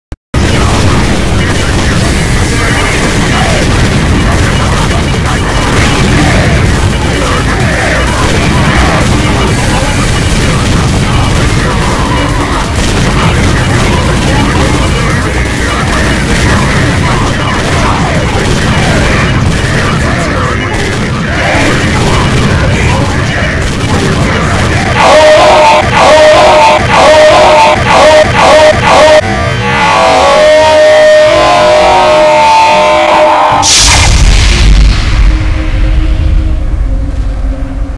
You're Going To Be Stupid And...
Yeah, yeah. I know. It's a shitpost. Just a little update on my channel. Yes. I know. I haven't uploaded in a long while. To be honest, I needed a break from CF, recordings and whatnot. Play other games and just catch up with myself. Just several other things I need to take care of in my personal life. Don't worry. I'll be back soon.